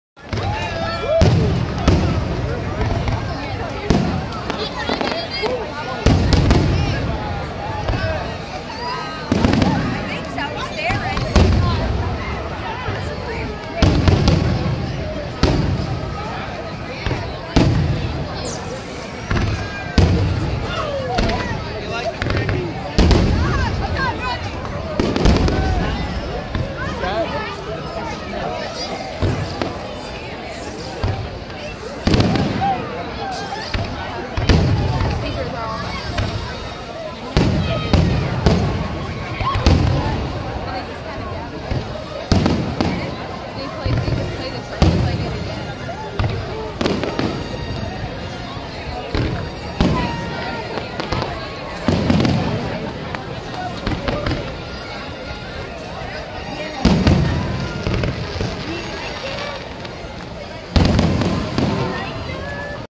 4th of July Fireworks
Funny Unicycle Kid had so much fun watching fireworks in Wildwood! They were so close to Drew. I hope you like them.